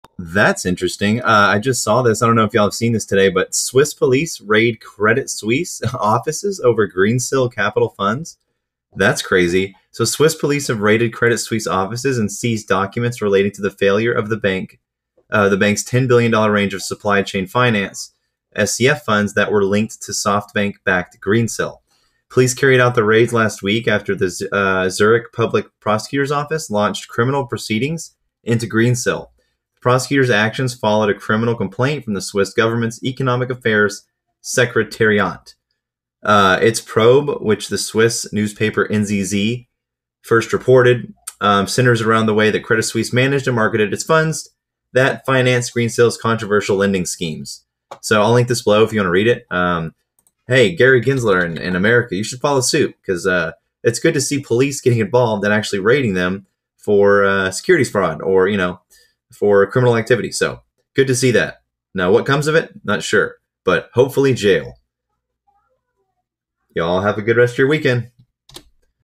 CREDIT SUISSE RAIDED BY SWISS POLICE!
LOL CRIME!

Is there actually going to be jail this time?

This is due to an investigation over fraudulent lending schemes.

Credit suisse?! No way!!